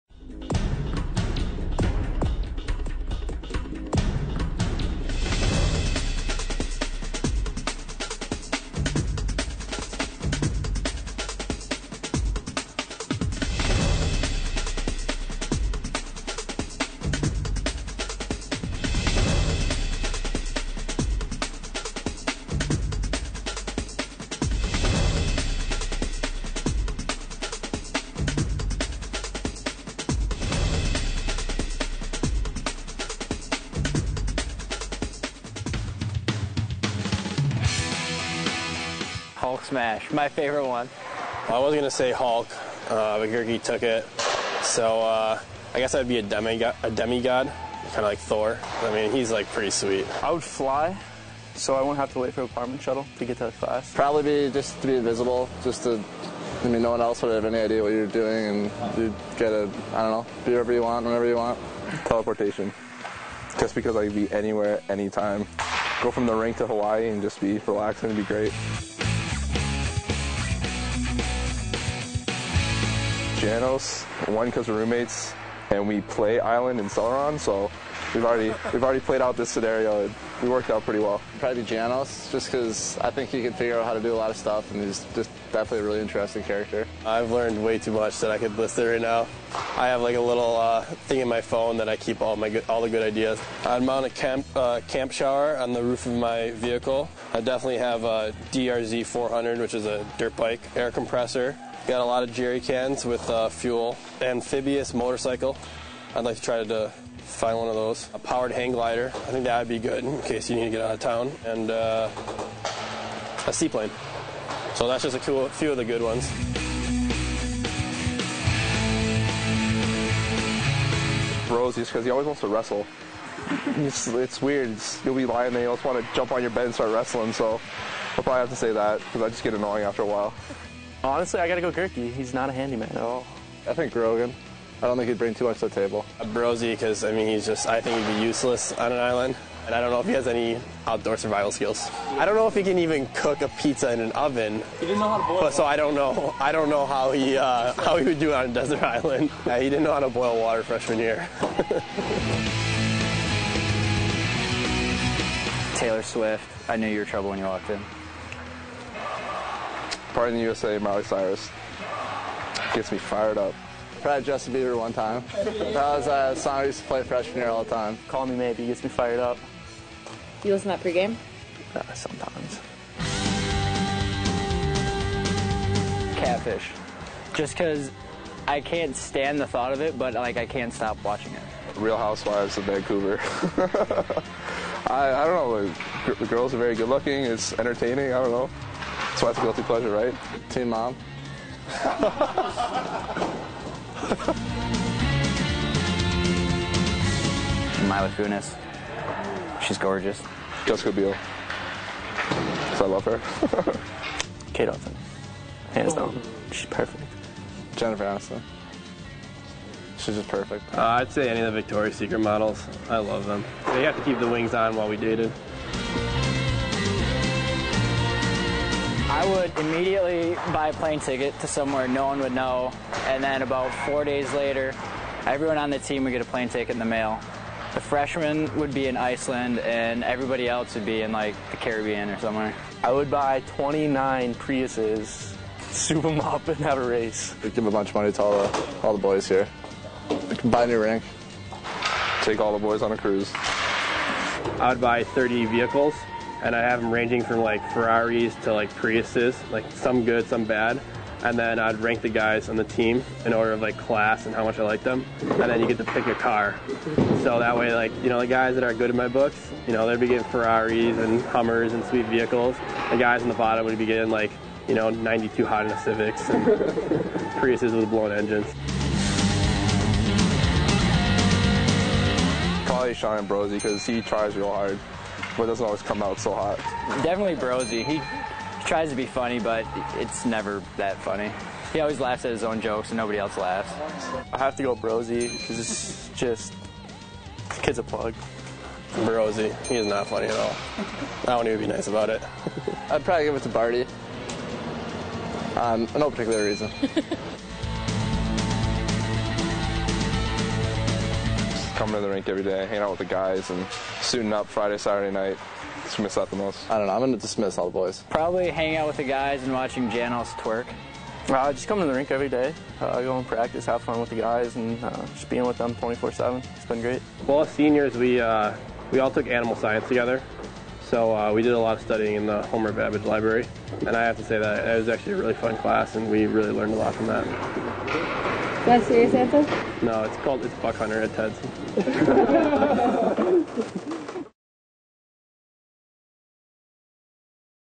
Men's Ice Hockey: Meet the Seniors
As the Huskies gear up for Senior Day on Friday, March 1 at 7:05 vs Sacred Heart, meet the 5 seniors that lead this squad.